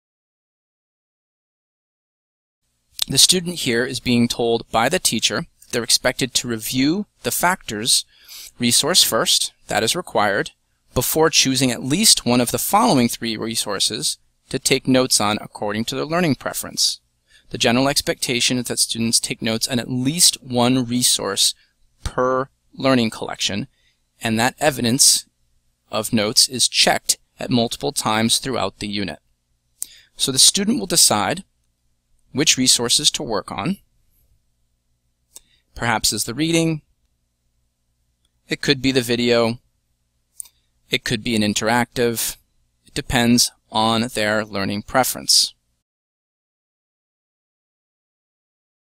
Student Choice of Learning Resources at LPS
How a student chooses learning resources for a learning objective